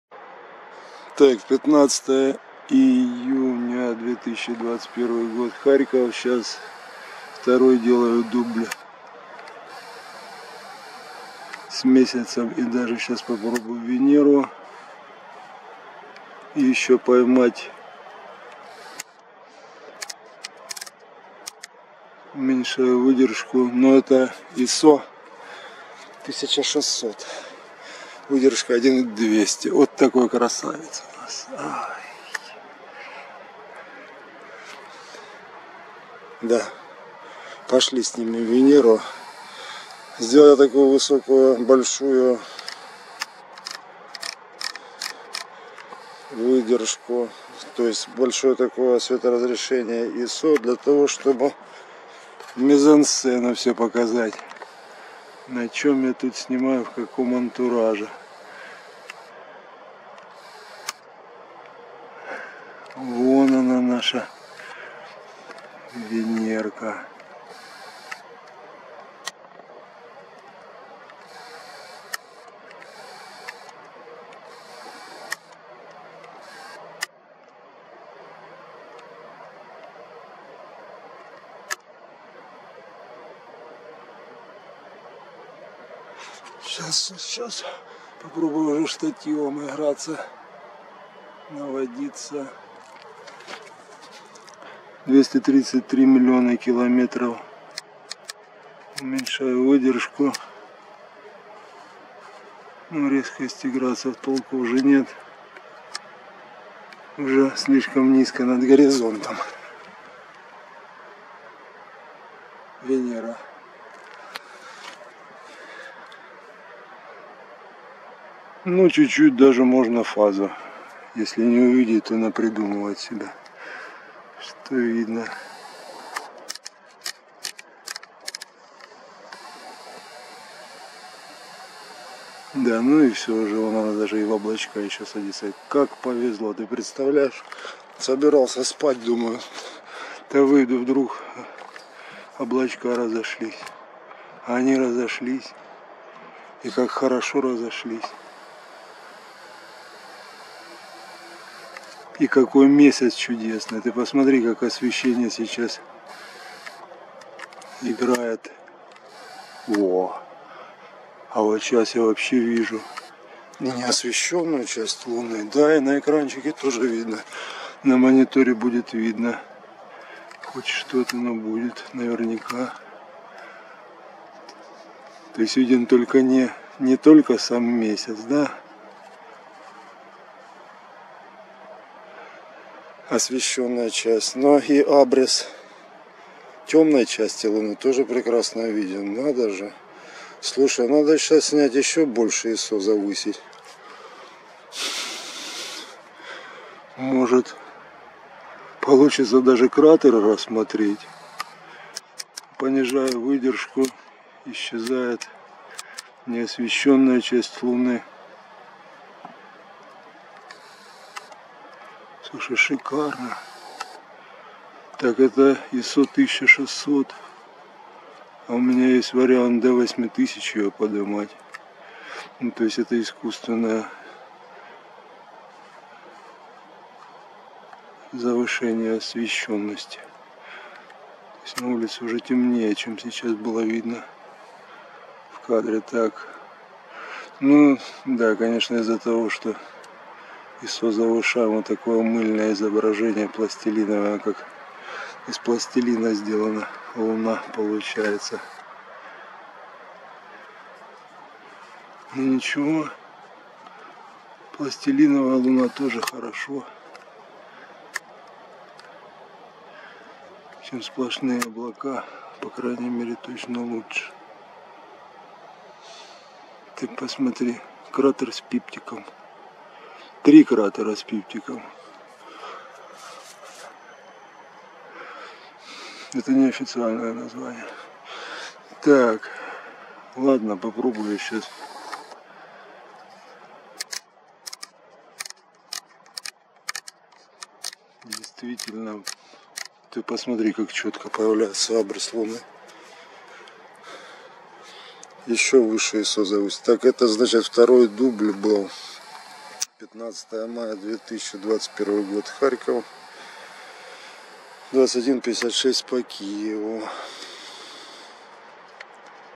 Мультяшный Месяц и Венера на закате 15 июня 2021 над Харьковом
Киношного вида Месяц опускается к горизонту в Харькове 15 июня 2021 года - на закате ещё видна Венера.